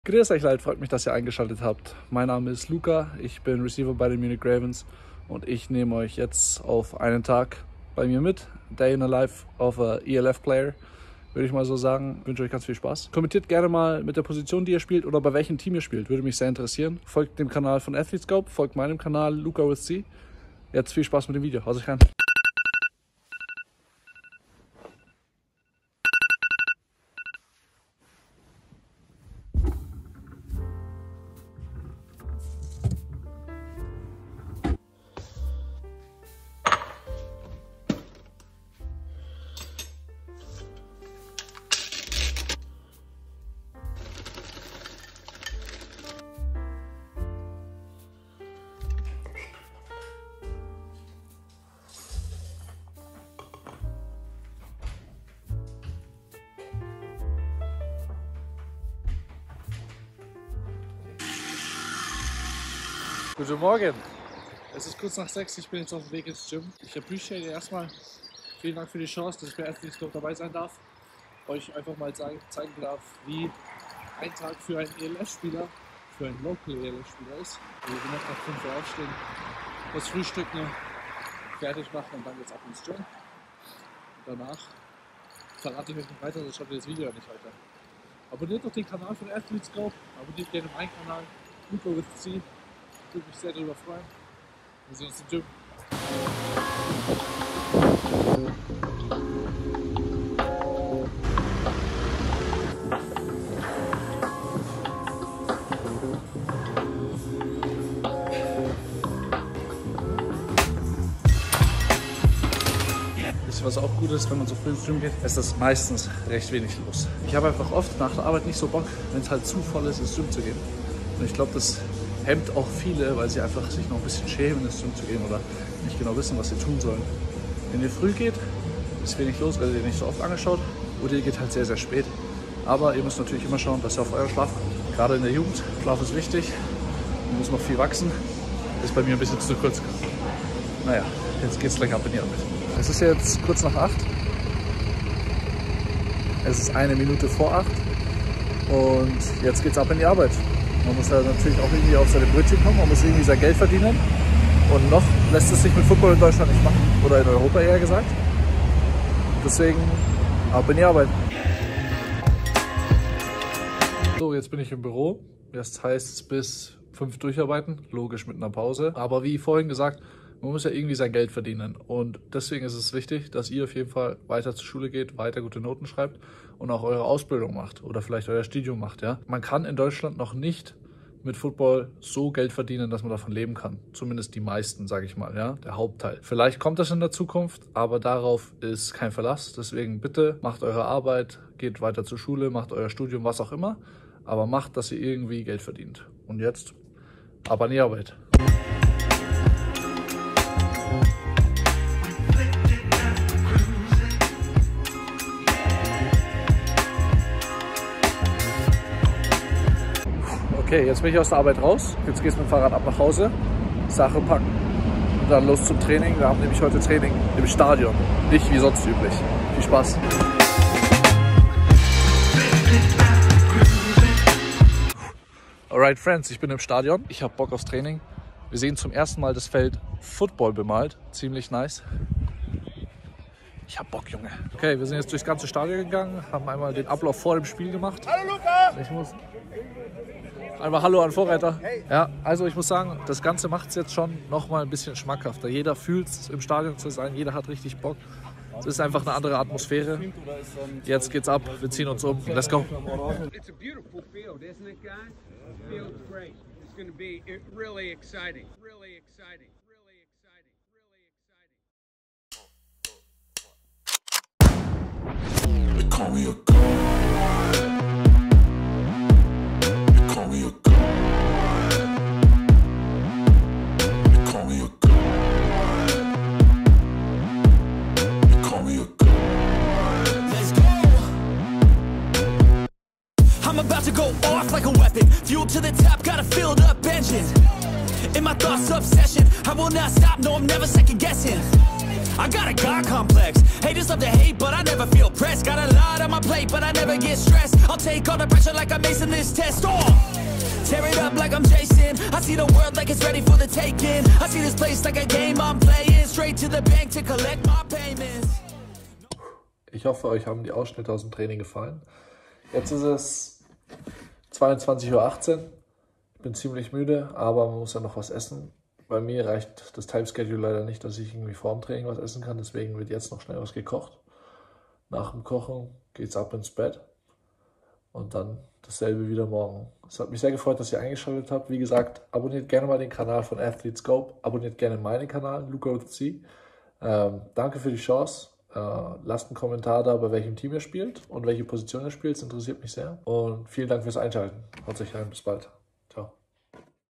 Day in the Life von einem ELF Spieler (American Football)
Dieses Video war für den Account von @athletescope gedacht. Durch ein langes hin und her, kommt es auf meinem Channel.
Beste Grüße gehen raus!

Danke an Viktor für die Trainingsausschnitte!